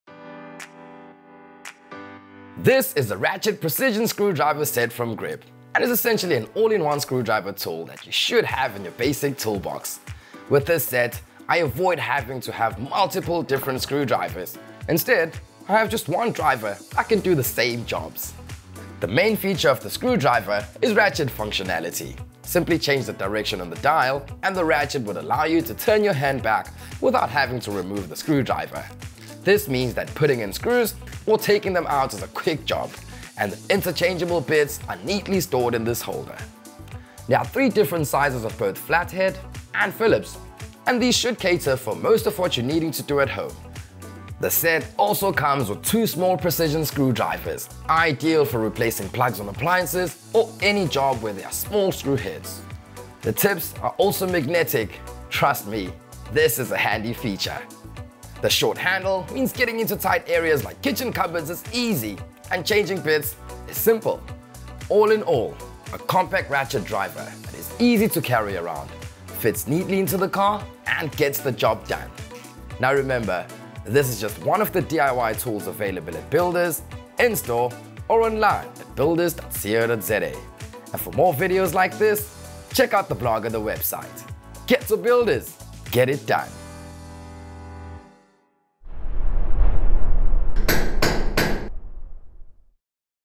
One Screwdriver That Does It All
Screwdrivers are an essential tool, everyone needs to avoid using the butter knife. The Grip GQ5356 9 Piece Ratchet Screwdriver Set is small and compact with all the essential driver bits, ideal if you don’t have space to store an entire set and your tool collection lives under the kitchen sink.